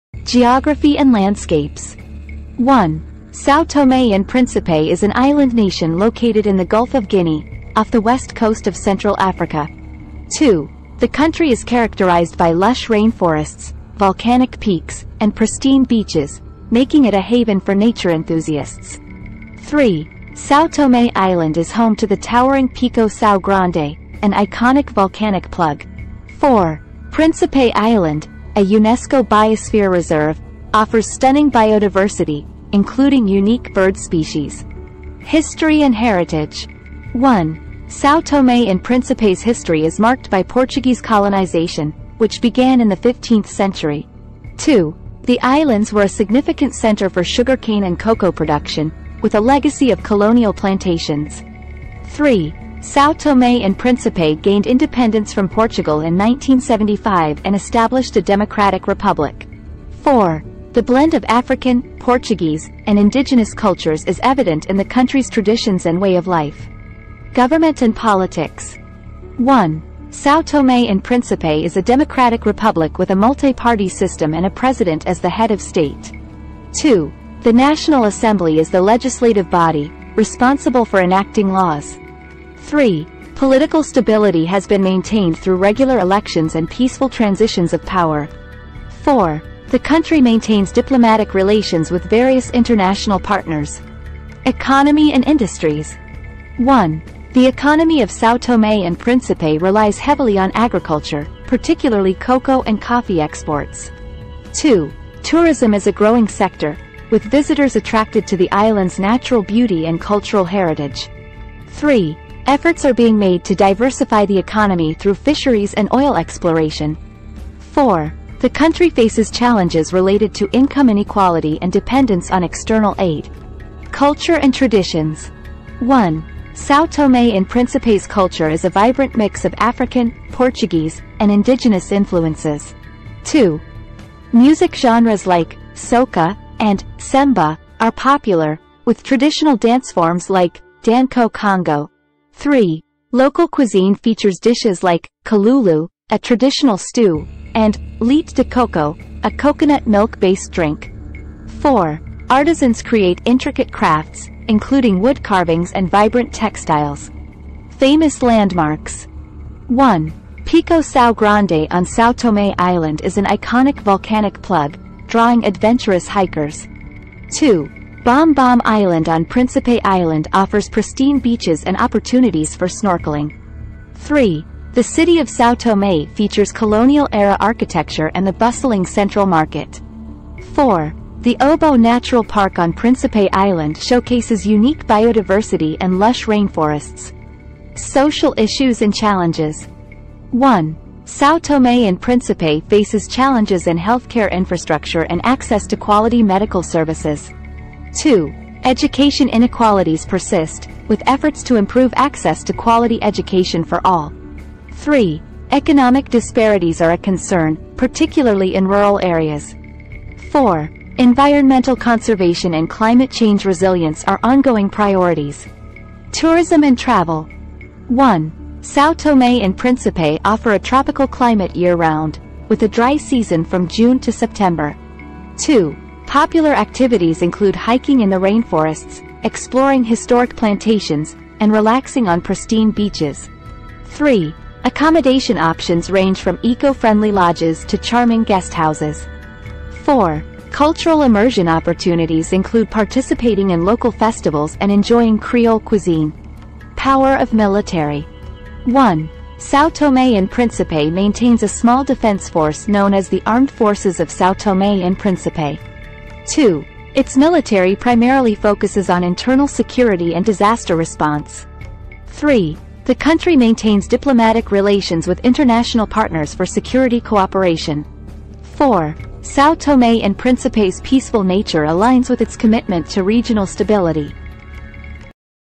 São Tomé and Príncipe: Africa's Hidden Tropical Paradise
Welcome to São Tomé and Príncipe, a tropical paradise off the west coast of Africa. In this captivating video, we'll uncover the breathtaking landscapes, delve into the rich history, explore the vibrant culture, and more that define this unique island nation. Whether you're planning your next adventure or simply curious about São Tomé and Príncipe, join us on this virtual journey to the heart of Africa's hidden gem.